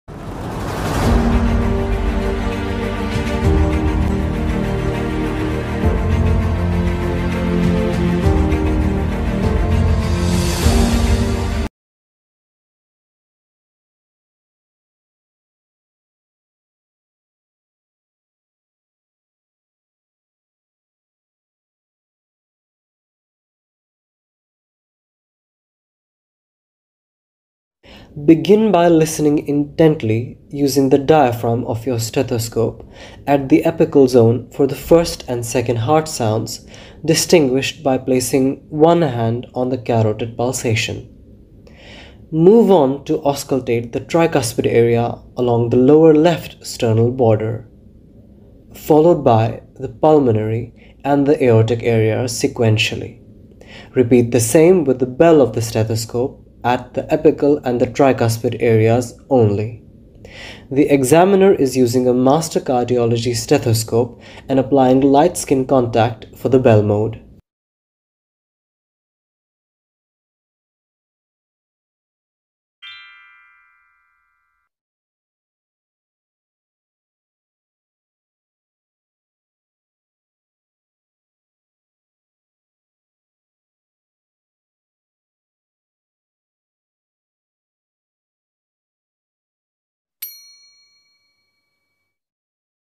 Auscultation - The Precordium | Standardized Clinical Examination Videos | KYAMC
Khwaja Yunus Ali Medical College (KYAMC) is proud to announce an online collection of Standardized Clinical Examination Videos for medical and dental students.

Demonstrated by: Dr. Aamir Amin, RU topper & Gold medalist (KYA-12 Batch)
Edited by: Noor Mohammad Al Murad (KYAD-01 Batch)